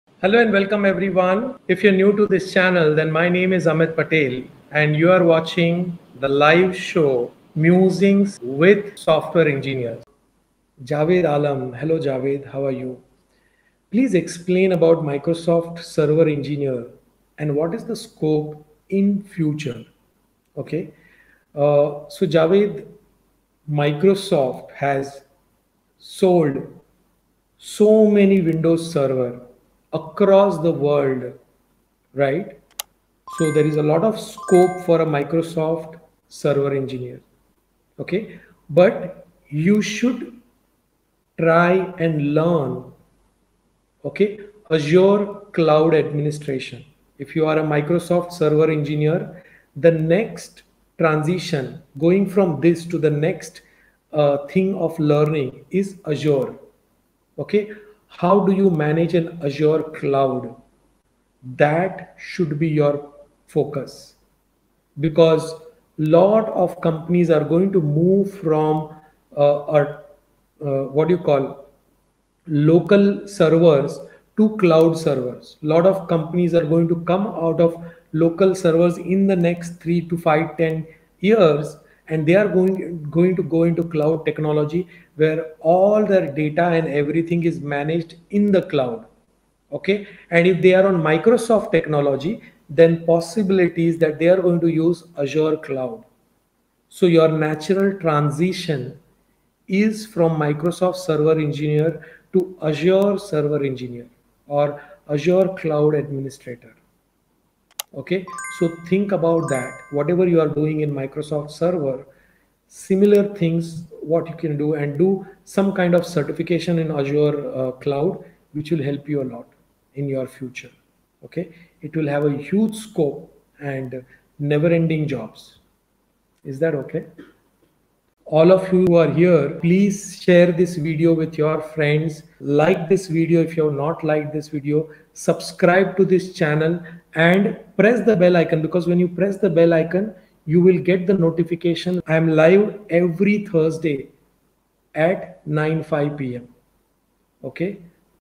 What is the scope and future of Microsoft Server Engineer?
Microsoft is a leader in the Windows Server Networking domain. Many engineers work on administration jobs on this server and operating system. In this video, we will discuss what is the scope and future of these network engineers who are administrators of Windows Server?